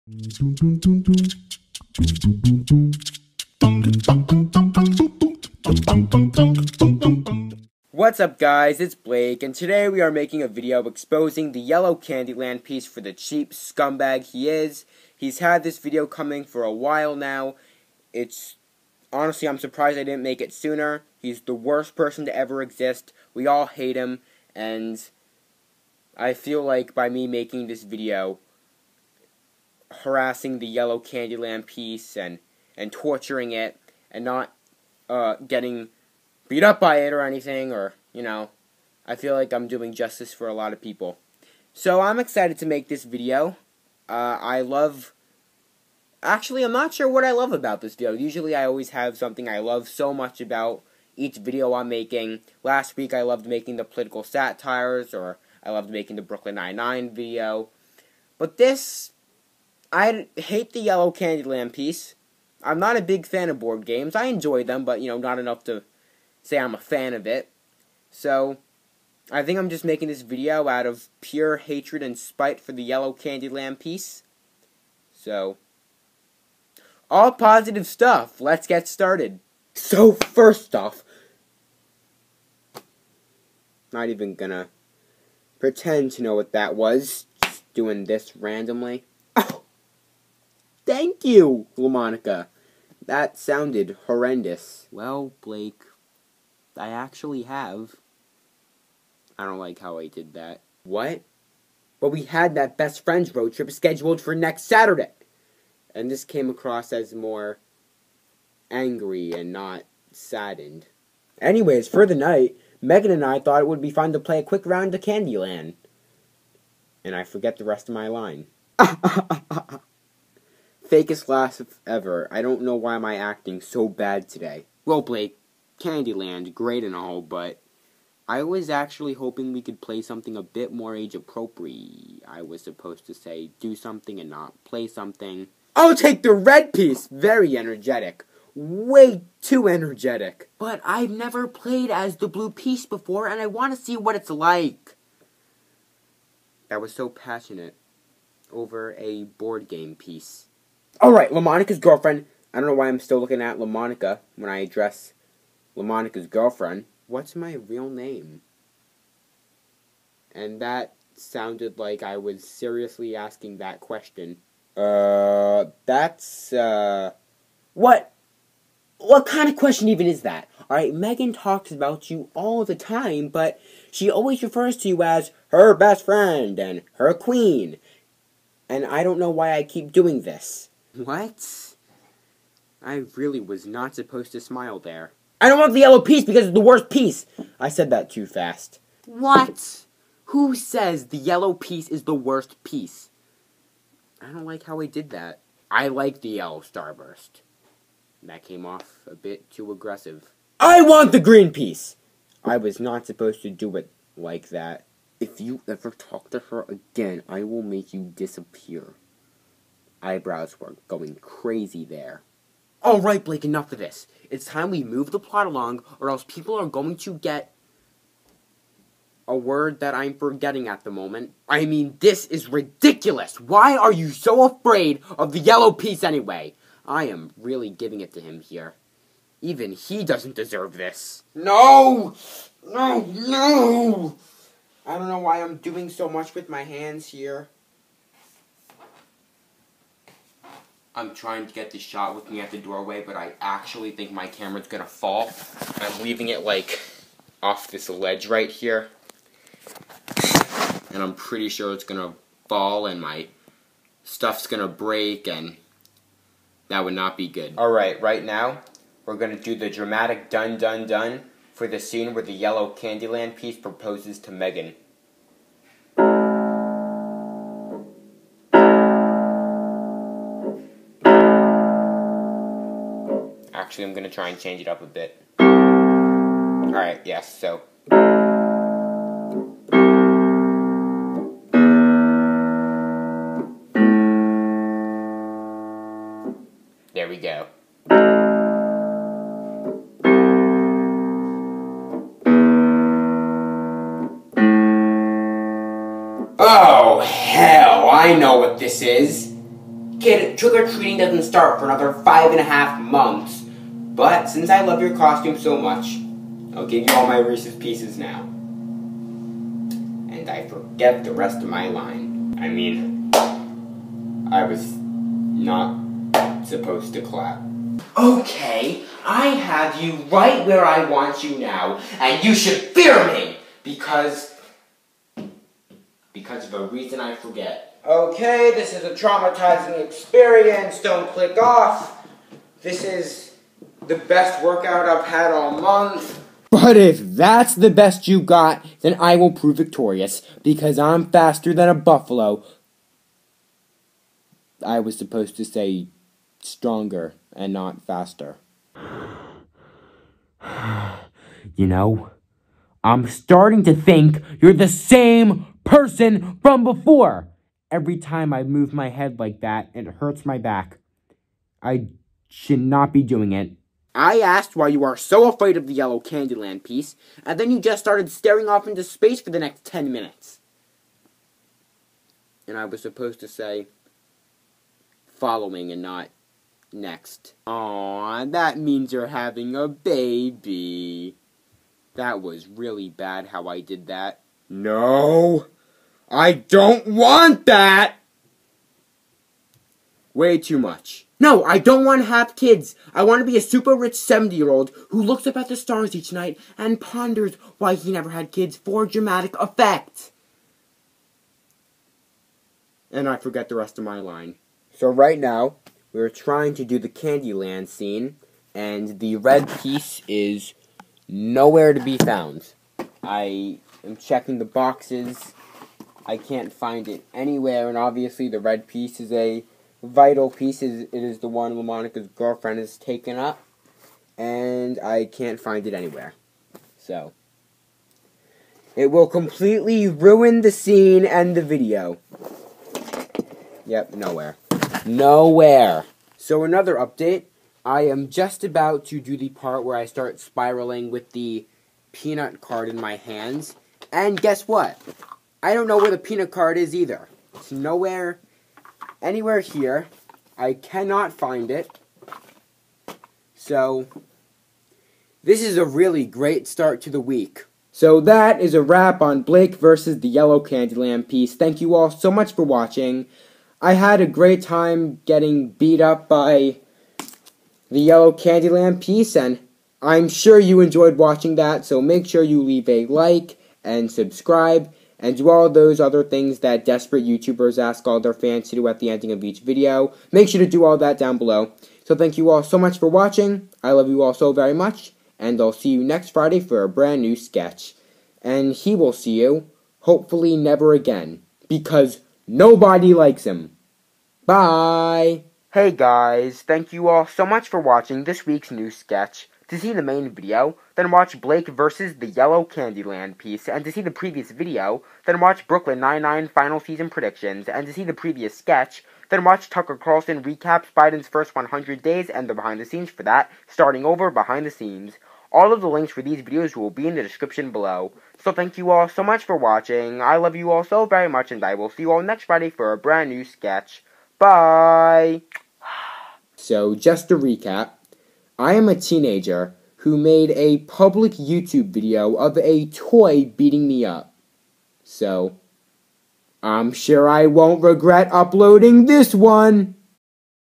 Missing Piece! (BTS)
Bloopers and behind the scenes for “Blake VS The Yellow Candy Land Piece!”